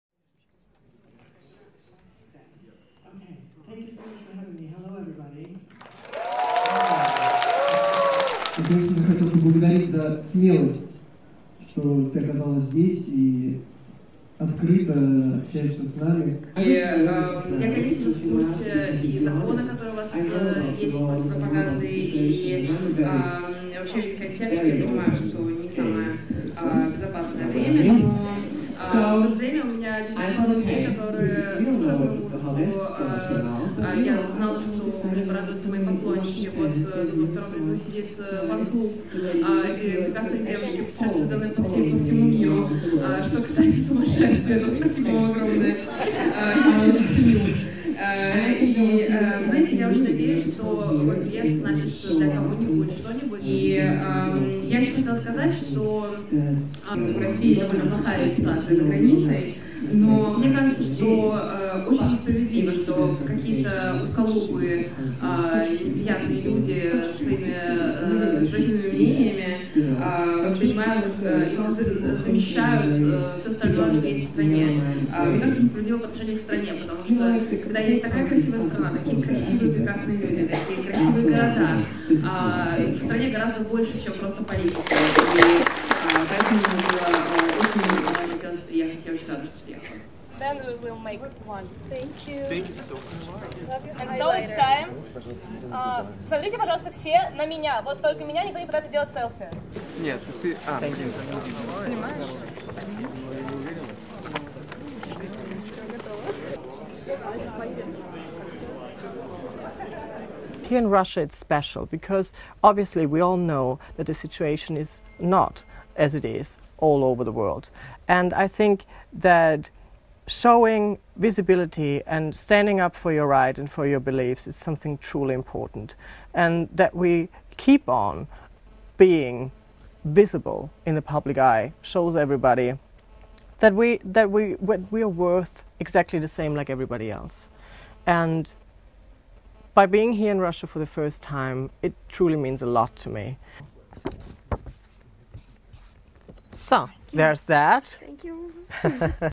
Кончита Вурст выступила в Петербурге
В Петербурге 25 ноября завершился 10-й ЛГБТ-кинофестиваль "Бок о бок". На церемонию закрытия приехала победительница конкурса "Евровидение-2014" Кончита Вурст.